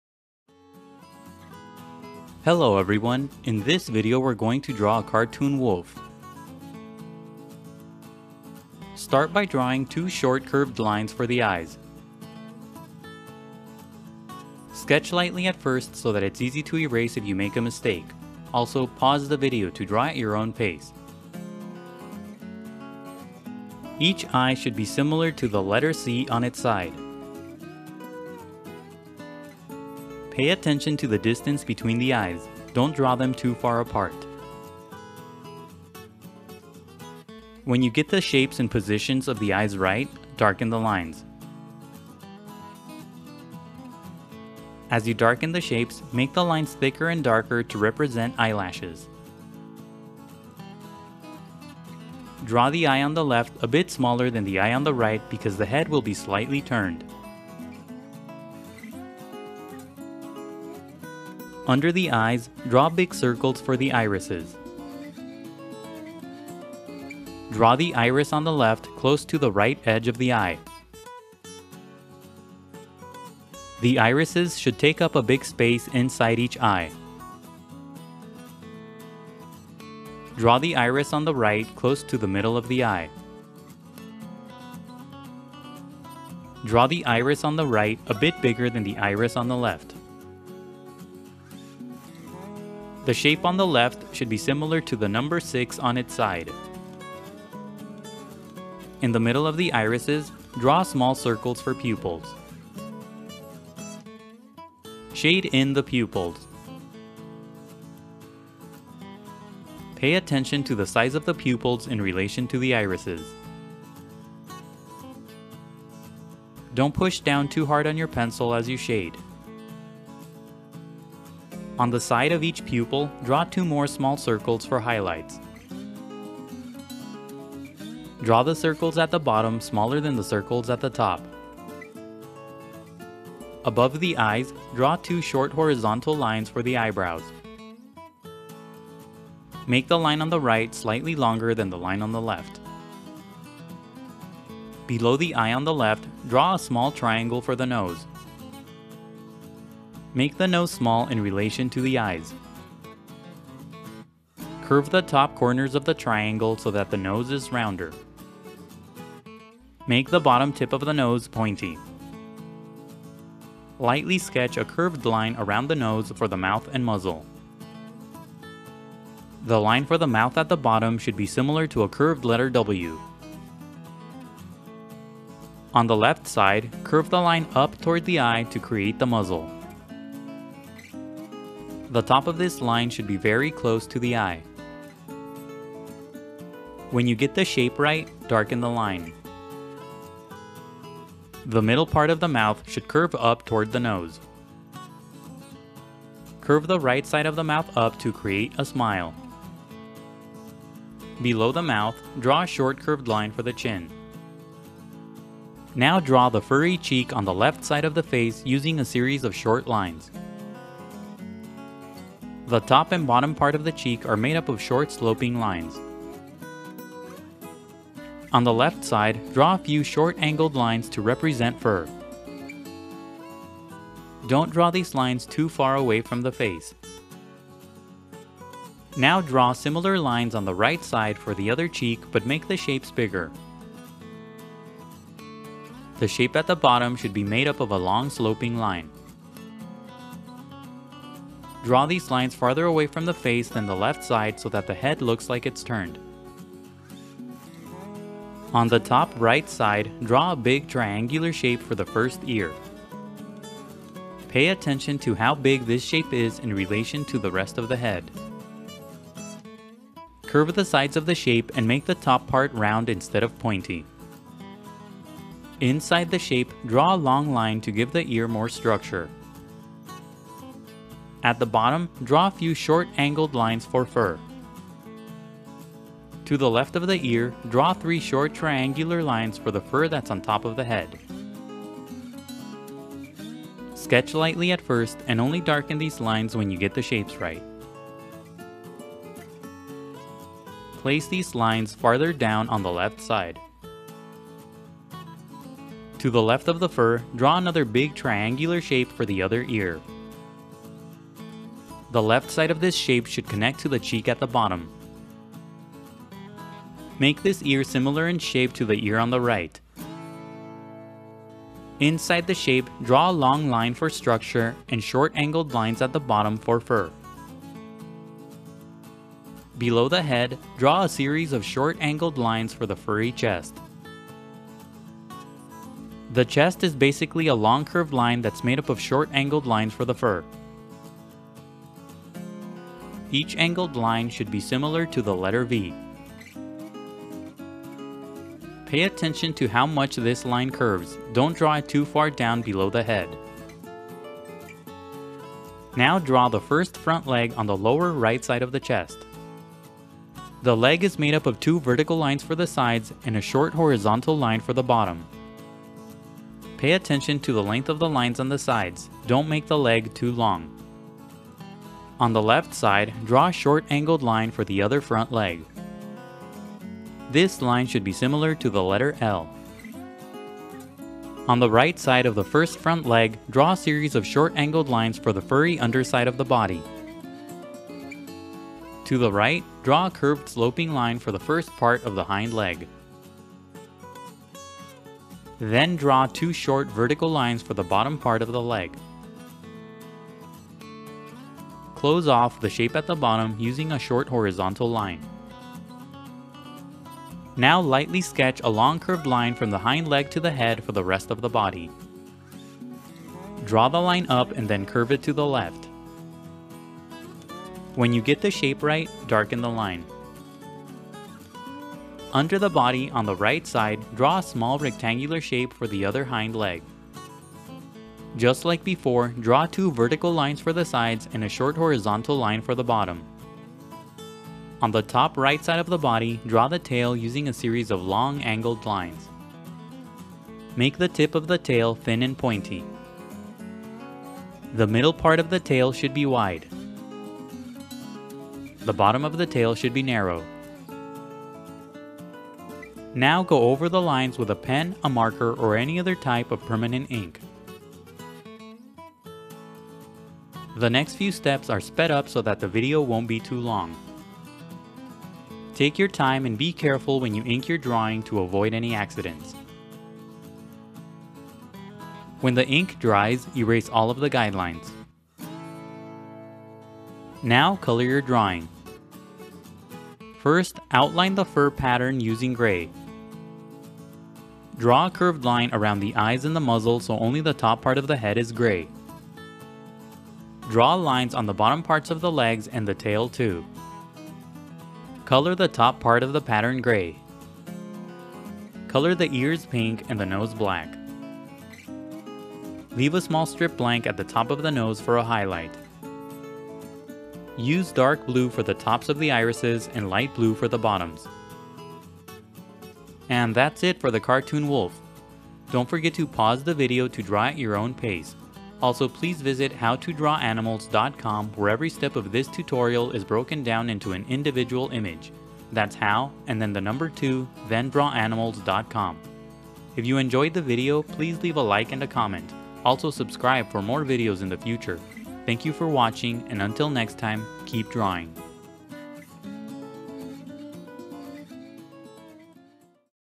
How to draw a Wolf (Cartoon)
This is a simplified cartoon version of a Wolf.

where every step is broken down to an individual image for an even easier tutorial and don't forget to PAUSE the video after each step to draw at your own pace.